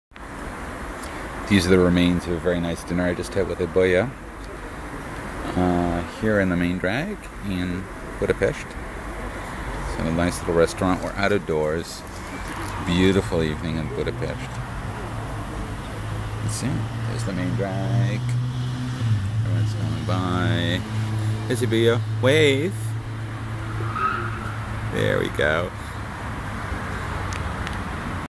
Ibolya at dinner
Ibolya and I dined at a very nice restaurant in Pest on Sunday evening; in the gloaming, I shot a little footage.